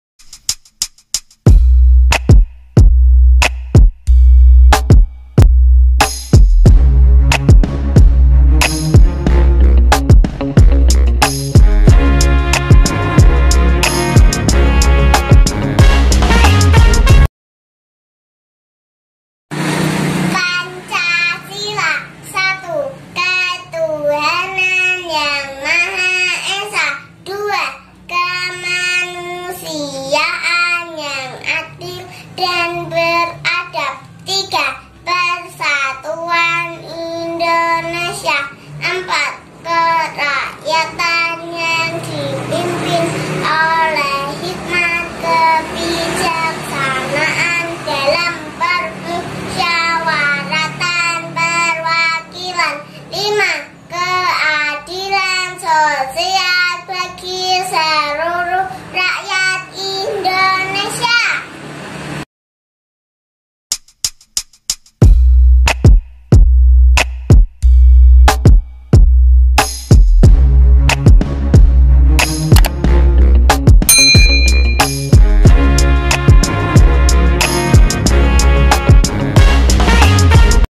PANCASILA ANAK PAUD
PANCASILA INDONESIA

Dibacakan oleh Nazwa Muslimmah
5thn